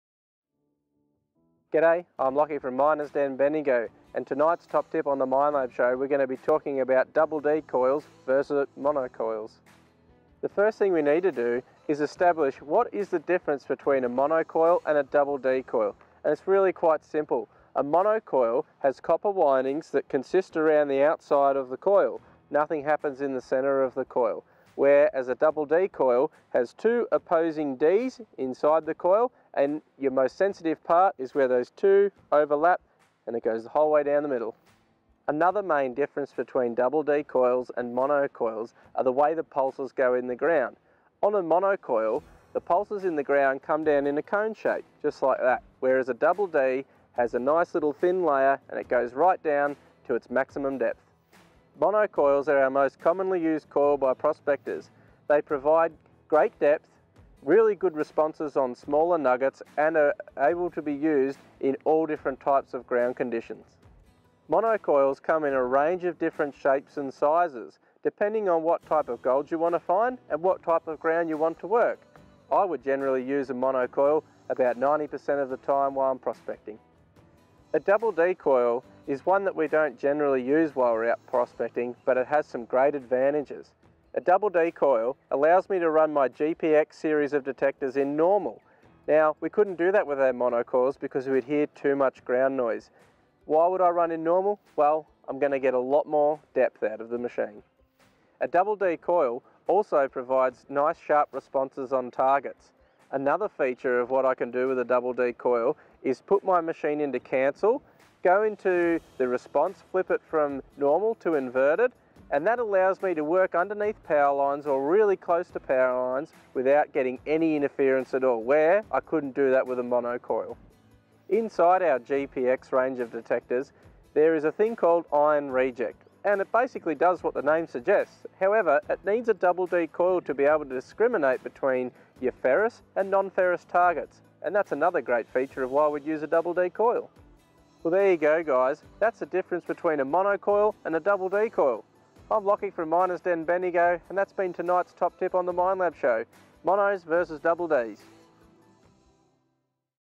Mono vs Double D Coils - Metal Detector Coil Comparison - The Minelab Show #37
Learn about the differences between Mono and Double D Metal Detector Coils.
Find out which coil you should use to find more targets with your metal detector.

For all the latest Minelab products and industry news.